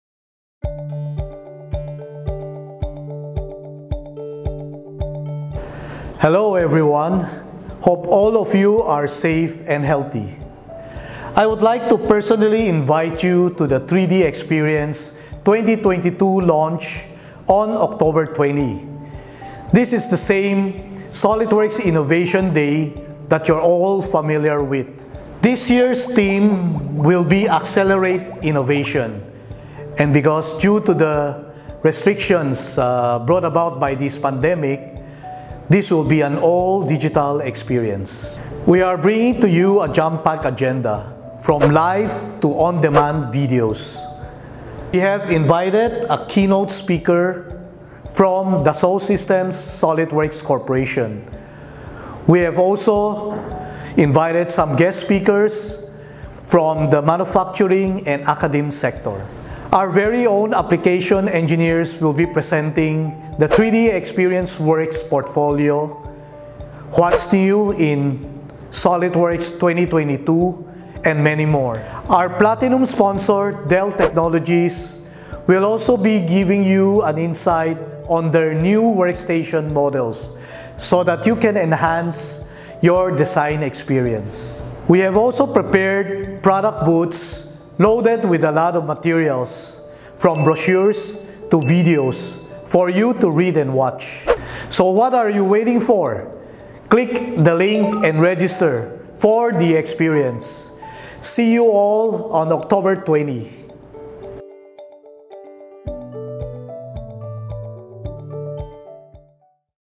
Our Managing Director Invites You to Computrends' 3DEXPERIENCE 2022 Launch | Oct 20, 2021
Computrends' Managing Director, Mr. George Ong personally invites you to the 3DEXPERIENCE 2022 Launch: Accelerate Innovation live on October 20, 2021.